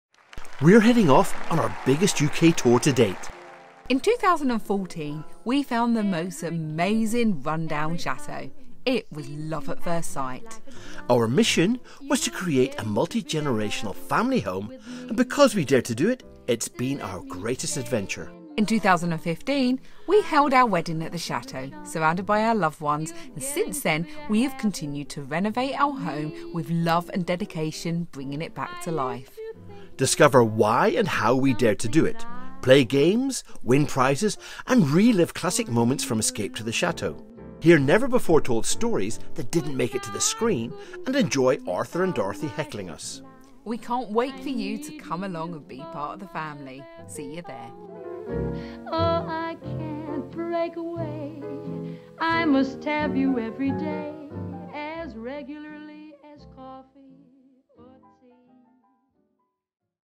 Dick and Angel: Forever Home (Avec Le Nitty Gritty) 2024 | Hall for Cornwall | 12 Nov | BOOK NOW
Spend an entertaining evening with the stars of Channel 4 hit Escape to the Chateau, Dick and Angel Strawbridge, and experience the Chateau like never before.

Over the past eight years, millions of viewers across the world have been enthralled by the adventures of retired Lieutenant Colonel, engineer and chef, Dick, and his entrepreneur and designer wife, Angel. Now, following two sell-out UK tours, the pair are heading back out on the road, sharing even more of the adventures, challenges, and successes of building and living their Chateau dream.

For their biggest tour to date, the couple will explore why they dared to do it, taking the audience behind-the-scenes of their forever home and what it takes to live your dreams. From the beginning of their Chateau journey to the present day, they will revisit classic moments and tell never-before-heard tales.

They may be an ‘odd couple’, but this charismatic pair have a love story to share and a lot to say. A highly interactive show, with hilarious games and the chance to pose your own questions, you do not want to miss this.